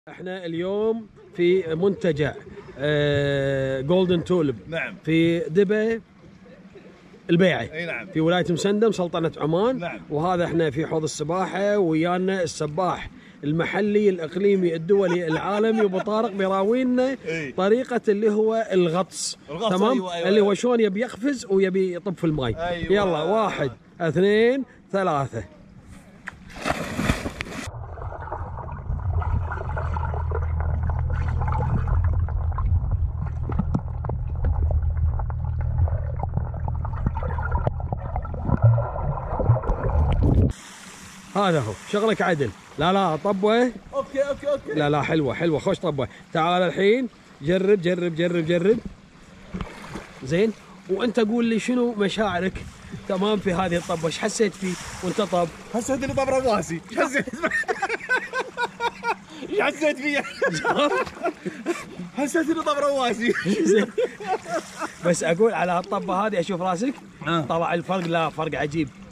سباحة وغطس في سلطنة عمان وتجربة جوبرو هيرو 6
gopro hero 6 underwater